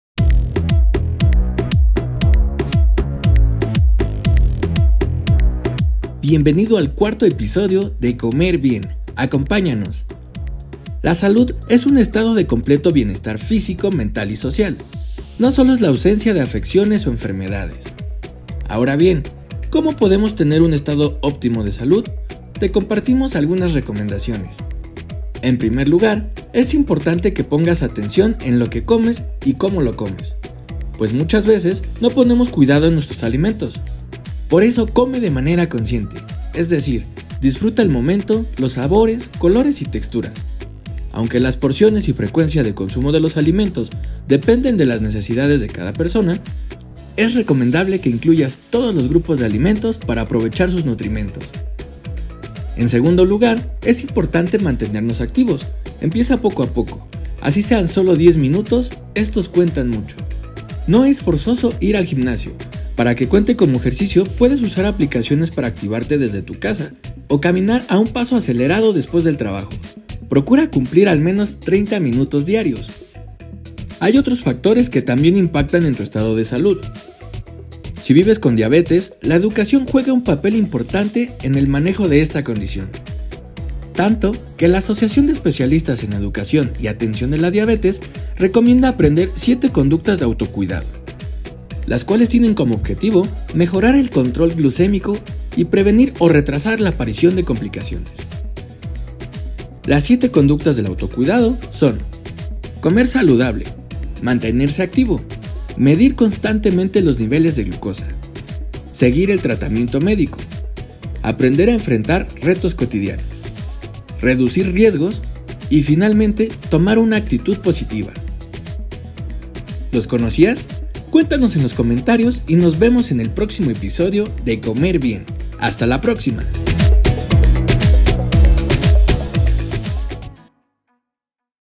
Episodio #4 Alimentación, ejercicio y educación como pilares de salud
Mantener un estado de salud óptimo no tiene que ser abrumador, si vives con diabetes no olvides acudir siempre con un médico especialista y un nutriólogo Educador en diabetes, pues de este modo podrás conocer más de la condición y tomar decisiones informadas para mejorar tu calidad de vida y disminuir el riesgo de complicaciones a largo plazo.

En voz de: Alejandro Sánchez Chavarría

X: fmdiabetes